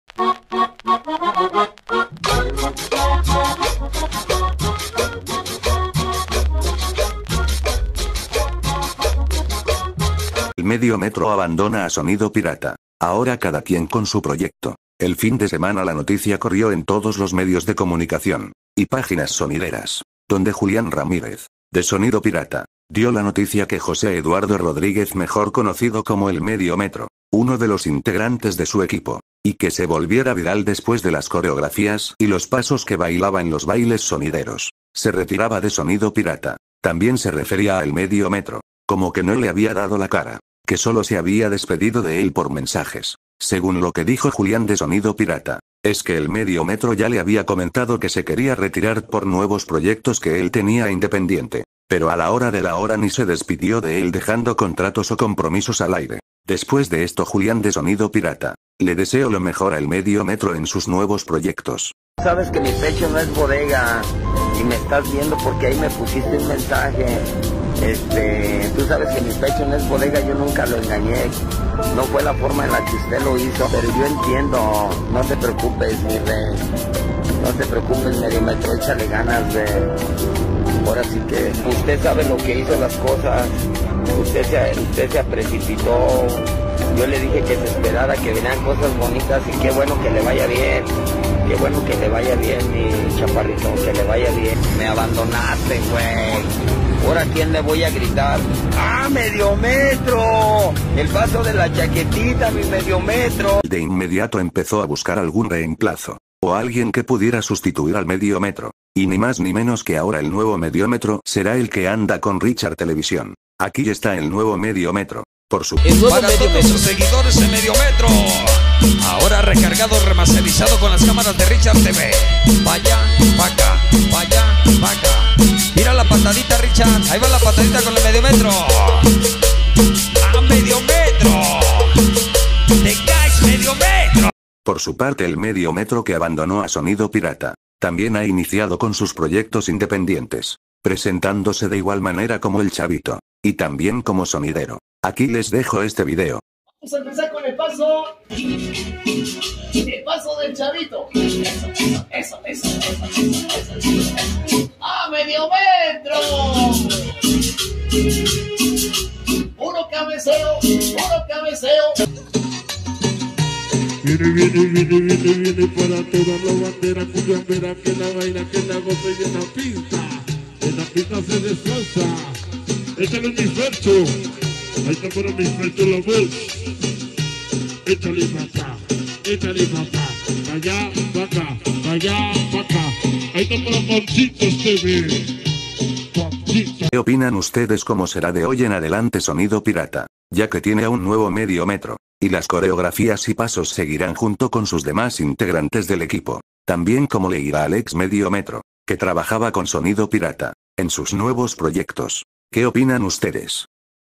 sonido pirata sin medio metro ahora cada quien con su proyecto nuevo.
se separa medio metro de sonido pirata y ahora cada uno de ellos está haciendo su propio proyecto forjando su propio destino en sus carreras.